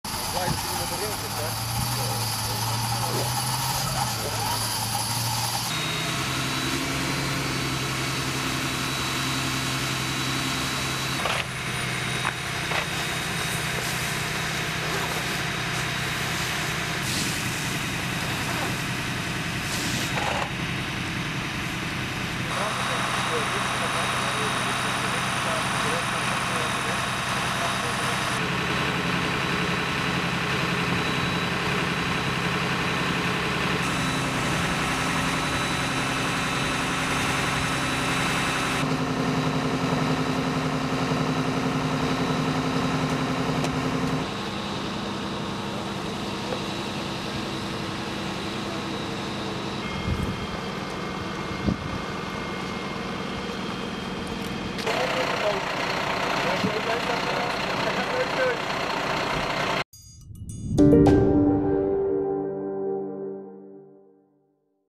Auto vliegt in brand tijdens het rijden in Almelo
Een auto is donderdagavond volledig uitgebrand op de Weezebeeksingel in Almelo. Door de autobrand is de weg in beide richtingen afgesloten voor al het verkeer.